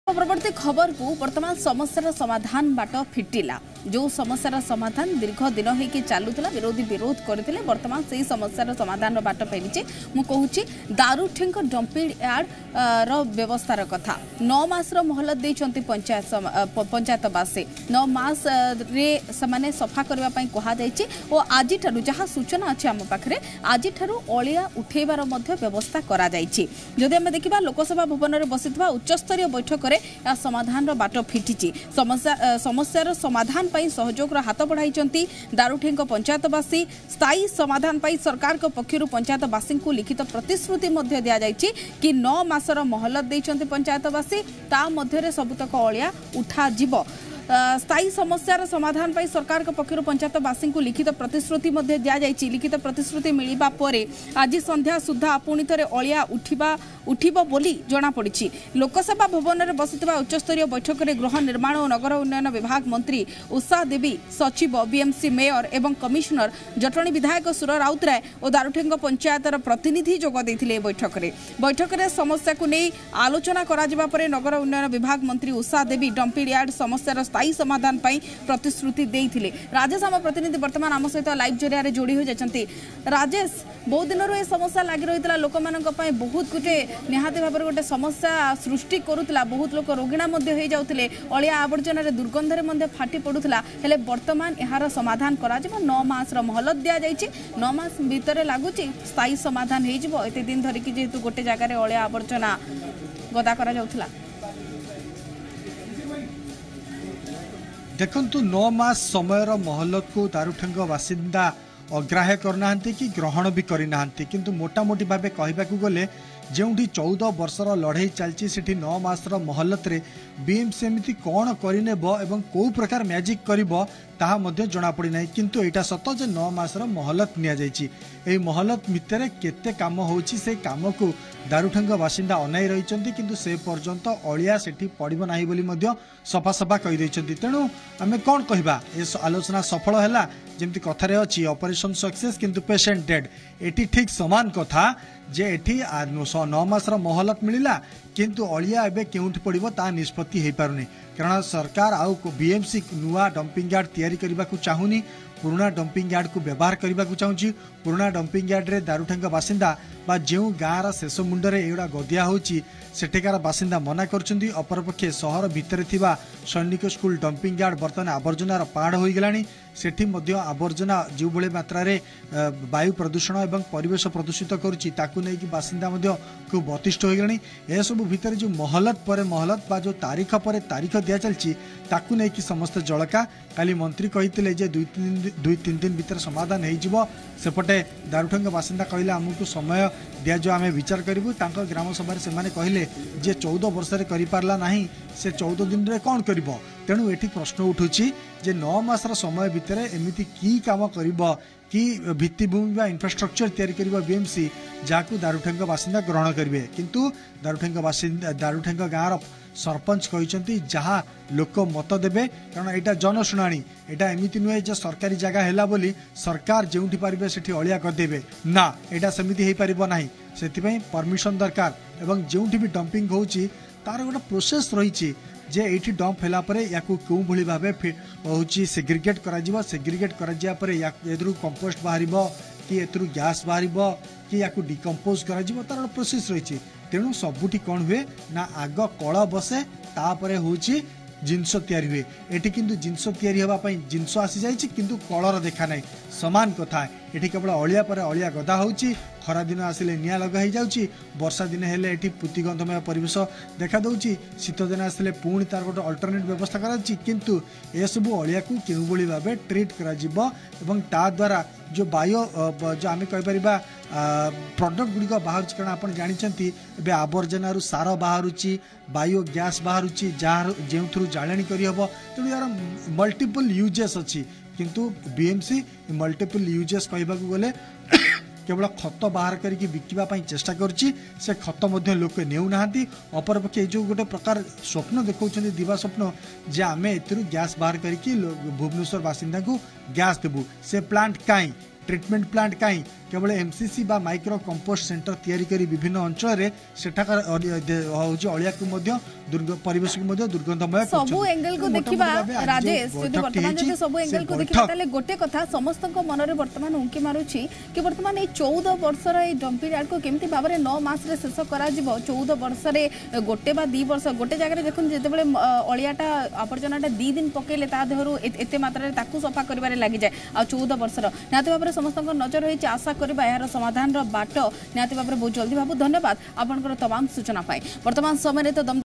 Daruthenga Dump Yard To Be Clean Soon | ଦାରୁଠେଙ୍ଗ ଡମ୍ପିଂୟାର୍ଡ଼ ରୁ ଅଜୀଠୁ ଉଠିବ ଅଳିଆ, BMCକୁ 9ମାସ ମହଲତ
ଦାରୁଠେଙ୍ଗ ଡମ୍ପିଂୟାର୍ଡ଼ ରୁ ଅଜୀଠୁ ଉଠିବ ଅଳିଆ, BMCକୁ 9ମାସ ମହଲତ
About Channel:-